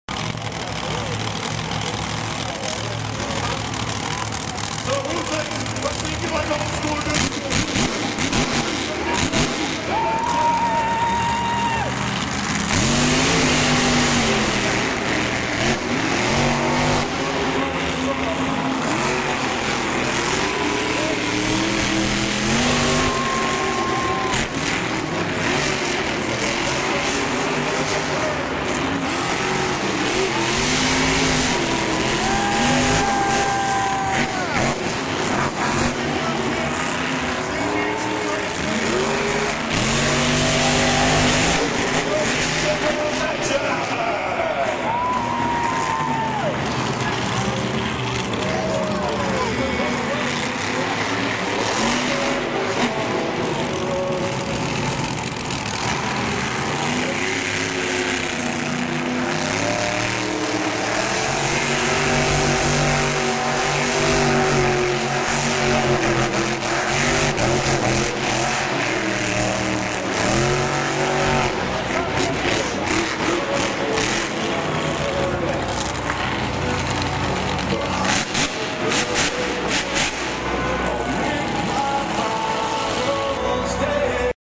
Monster Jam Philadelphia 2011- Championship Race
Grave Digger vs. Grave Digger the Legend.
Winner Grave Digger the Legend